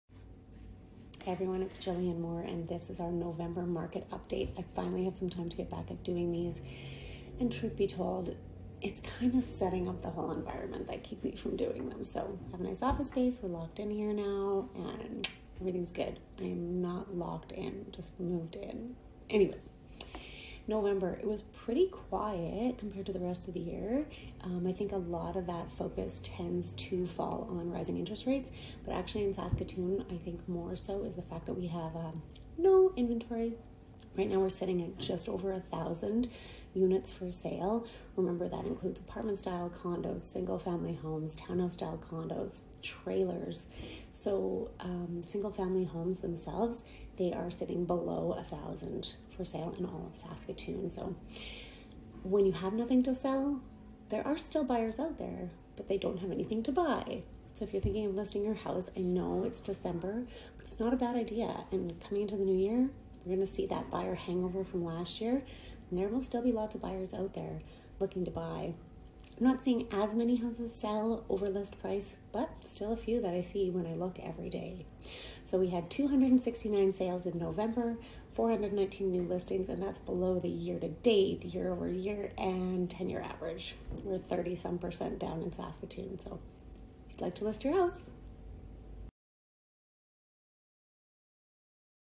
November Market Minute
All we are hearing about is interest rates. Yes, they’re up. But remember that the media’s real estate information is mainly driven by Eastern and (way) Western Canada’s markets - read Toronto & Vancouver.
I would like you to consider the fact that in Saskatoon we are starving for listings! When there’s nothing to buy it’s tough to reach the numbers in sales!
“November inventory levels fell to the lowest level reported in over a decade”
-SRA

🏡 Months of inventory: 3.8 Sellers Market
🏡 New Listings: 419 units
🏡 Total Inventory: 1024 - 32% below 10yr average
🏡 Benchmark Price: $367,800 ⬆️ 2.7%